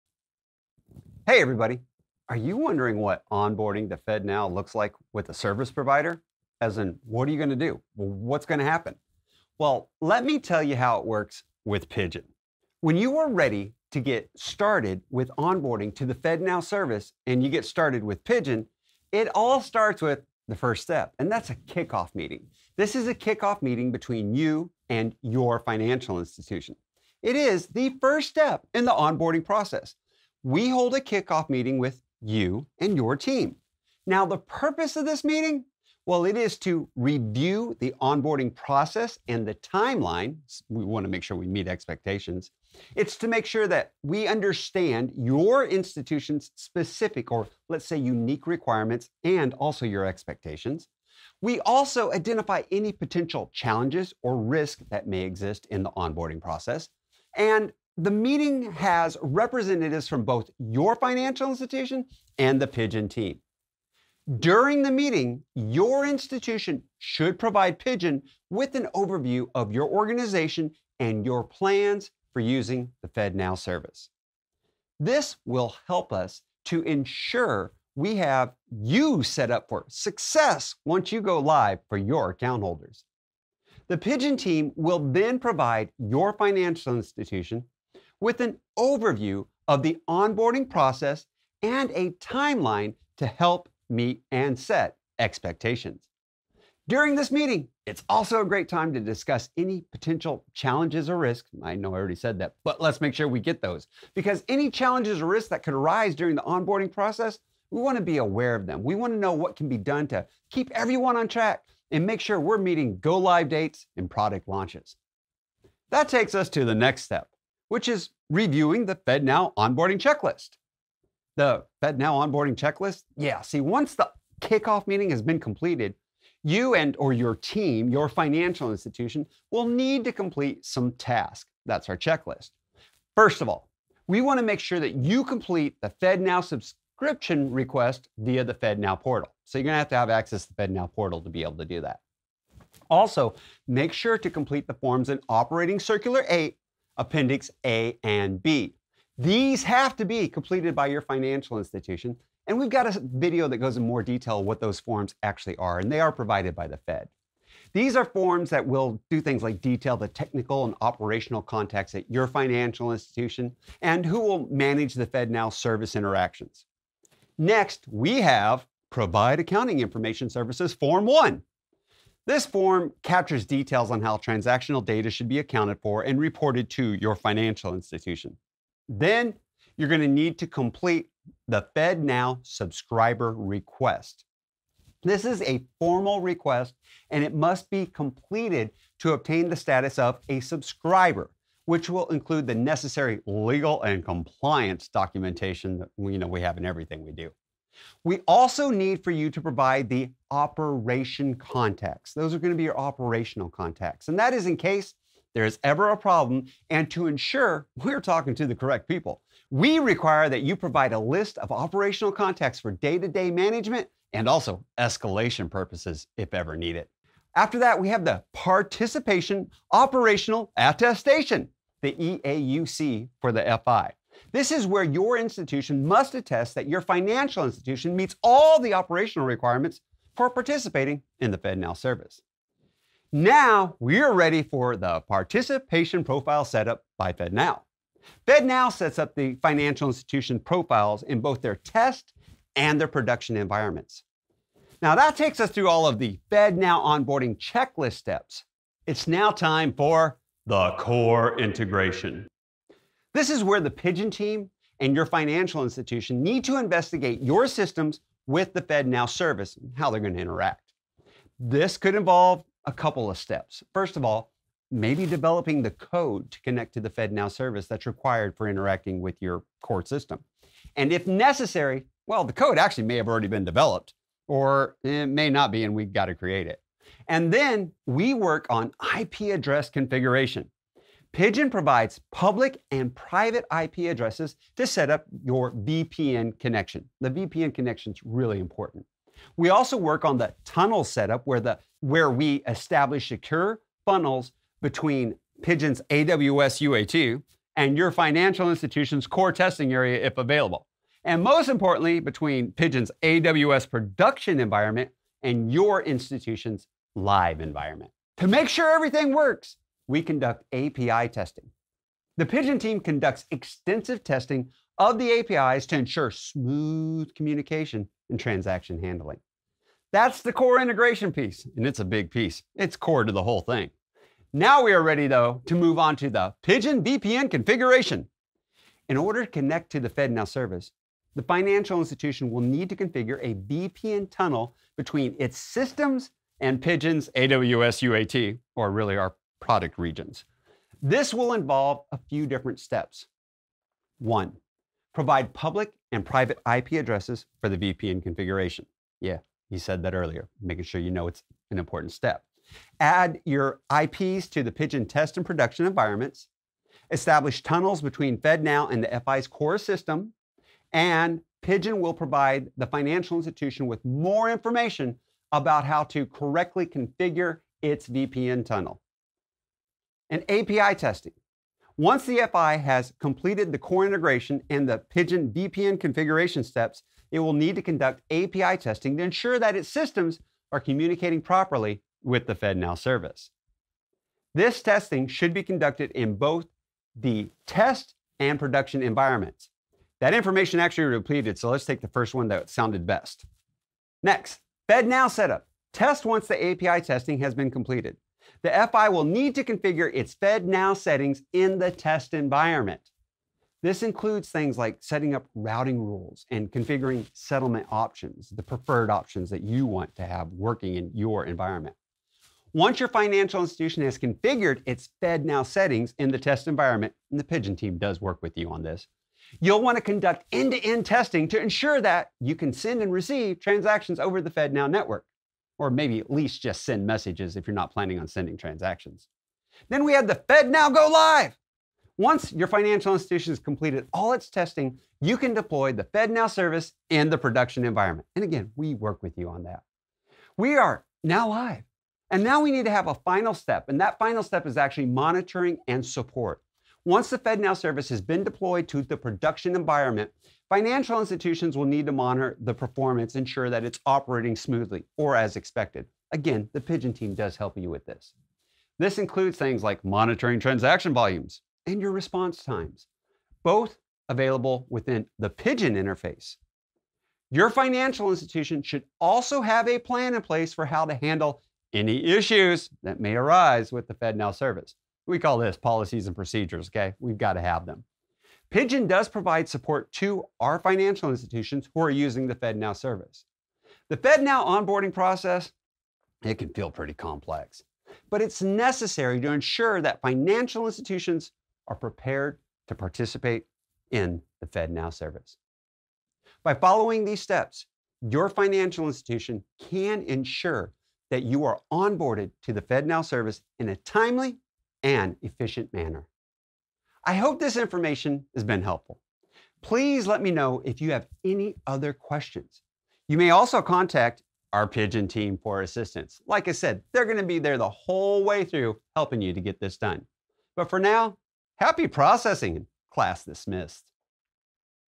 Everything you need to know about onboarding to the FedNow Service!
Are you a Bank or Credit Union looking to onboard to the new FedNow® Service?
Embarking on the FedNow® journey with Pidgin? Our latest video guides you through every step of the onboarding process to the FedNow service using Pidgin. We start with an essential kickoff meeting, where your financial institution and the Pidgin team come together to outline the onboarding process, understand your specific needs, and identify any potential challenges. This meeting sets the stage for a successful implementation, ensuring we align with your institution's vision and objectives for using the FedNow® Service. Next, we delve into the heart of onboarding with the FedNow® Onboarding Checklist, a comprehensive guide to ensure all necessary steps are covered. This includes completing the FedNow subscription request, filling out vital forms for operational contacts, providing accounting information, and setting up participant profiles in FedNow's test and production environments. It's a detailed journey, but we're with you every step of the way to make it as smooth and efficient as possible.

Moving to the core integration phase, we focus on aligning your systems with the FedNow service. This involves collaborative efforts in code development, IP address configuration, and establishing secure communication tunnels. We ensure thorough API testing to guarantee seamless transaction handling. The process culminates in the FedNow® Go-Live, where your institution starts operating in the production environment. But our support doesn't end there. We provide continuous monitoring and dedicated assistance to ensure the smooth operation of the FedNow service. Our video offers a clear and concise roadmap for integrating FedNow with Pidgin from initial planning to live operation. For further guidance or inquiries, our Pidgin support team is always available to assist you. Watch our video now and take the first step towards a transformative digital payment experience with FedNow® and Pidgin.

"We make learning about payments rules and regulations fun and easy!"